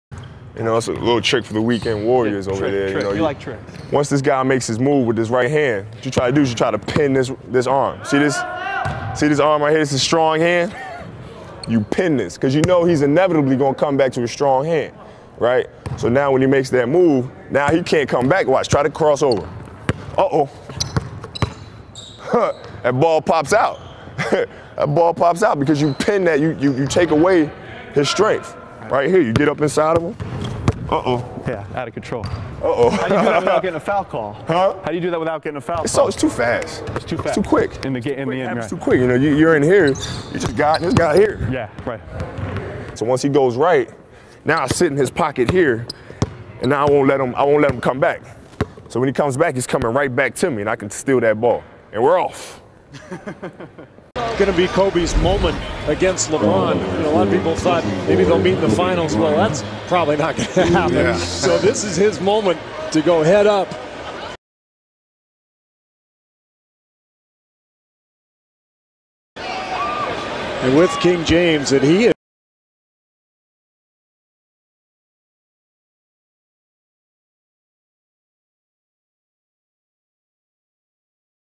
Kobe Bryant Perimeter Defense Technique:How to
Kobe Bryant is one of the smartest basketball players ever.

This video show's how Kobe pins his defender's arm and how that helps him on defense.